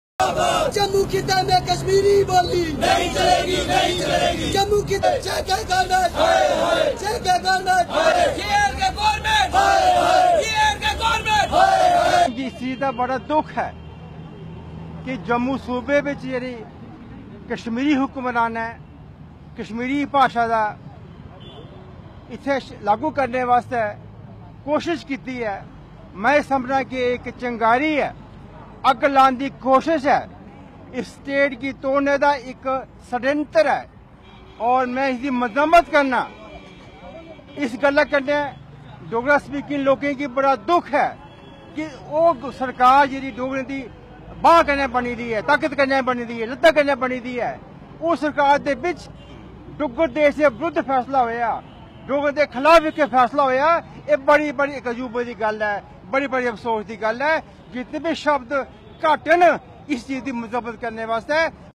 Protest march | Against Government's decision | Jammu
The Jammu and Kashmir government's decision to create 49 assistant professor posts for teaching Kashmiri in colleges of the Dogra-majority Jammu belt has triggered a row with various Jammu-based organisations terming the order as an "assault on Dogra identity". Several organisations, including the Dogri Bhasha Academy and the Jammu Duggar Manch, today took out a protest march against the move.